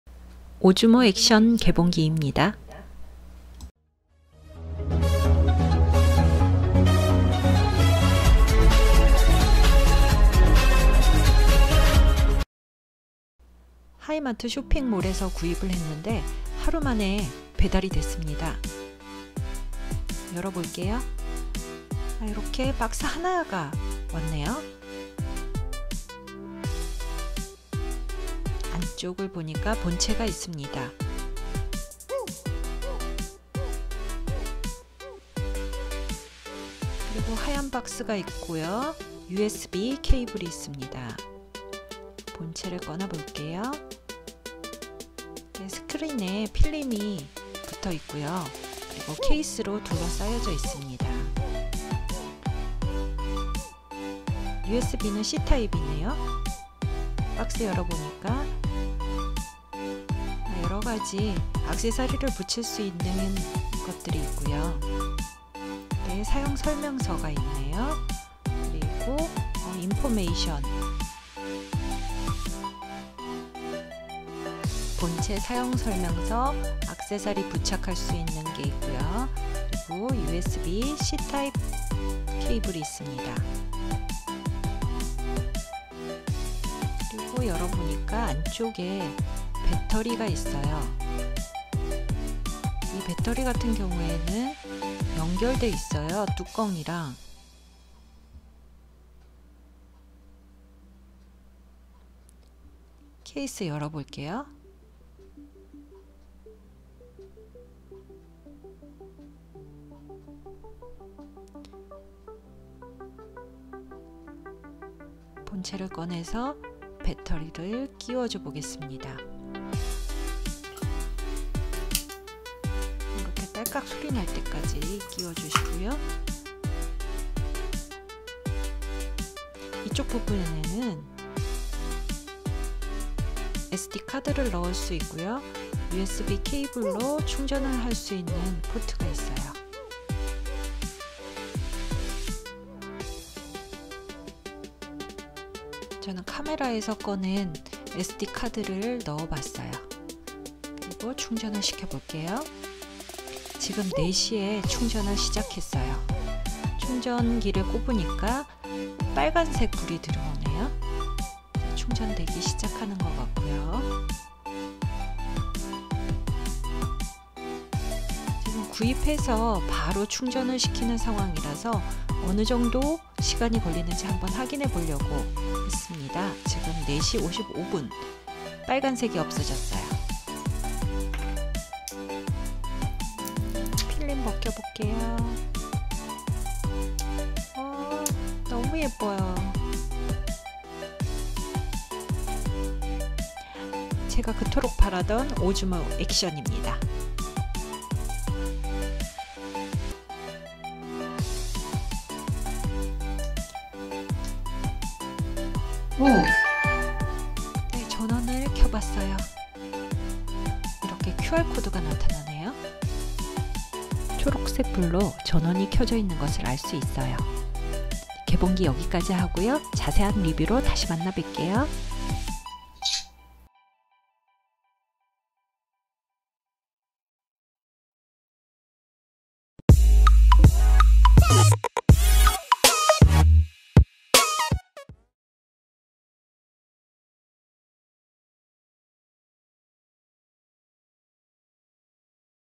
시니어유튜버 오즈모액션캠 개봉기 ㅣ 고프로? 오즈모? 셀피스크린에 반해서 구입한 DJI Osmo Action ㅣ 시니어봄tv
액션캠을 너무 가지고 싶어 고프로와 오즈모액션캠으로 며칠을 고민했지만 셀피스크린이라는 기능에 반해서 오즈모액션캠으로 결정했어요 먼저 개봉기 입니다. 리뷰영상도 곧 찍어서 올릴께요~
시니어유튜버의 새로운 도전을 응원해주세요.